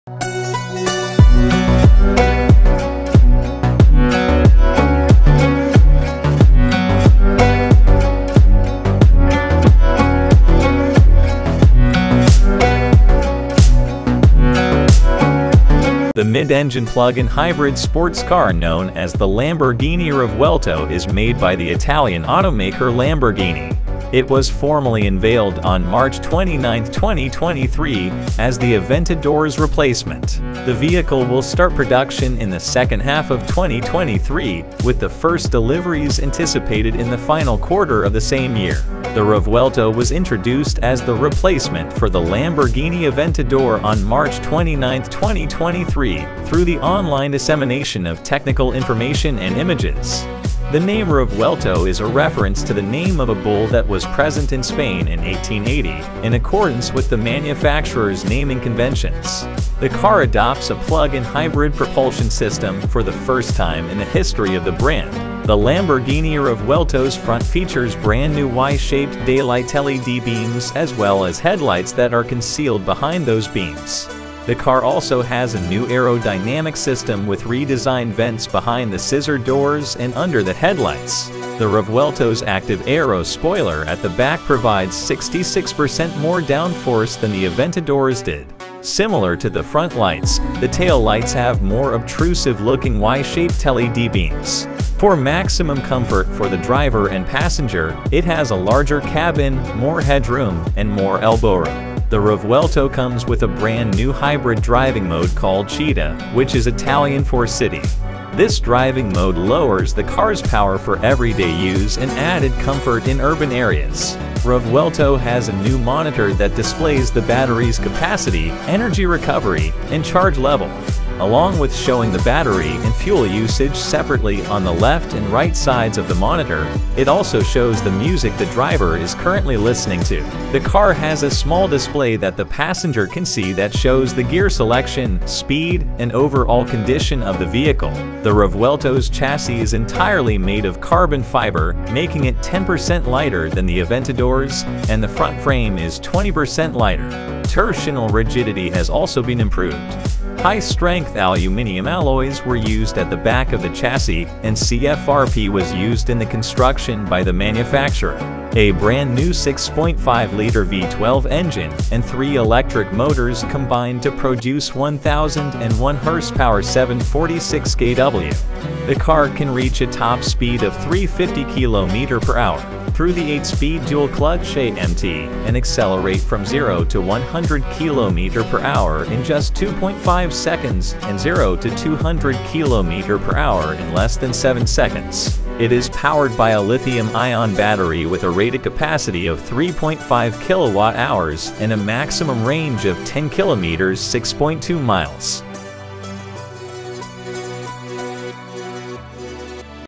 Lamborghini | Lamborghini Revuelto | Hybrid Lamborghini | lamborghini aventador
Get ready to witness the future of automotive design and performance with the Lamborghini Revuelto. In this video, we dive deep into the captivating world of the Lamborghini Revuelto, a groundbreaking concept car that pushes the boundaries of speed, innovation, and style. Join us as we explore its futuristic design, cutting-edge features, and revolutionary technologies that make it a true masterpiece on wheels. Prepare to be mesmerized by the sleek lines, jaw-dropping power, and unmatched driving experience that the Lamborghini Revuelto offers. Don't miss this exclusive glimpse into the next generation of supercars. Buckle up and join us on this thrilling ride into the world of the Lamborghini Revuelto.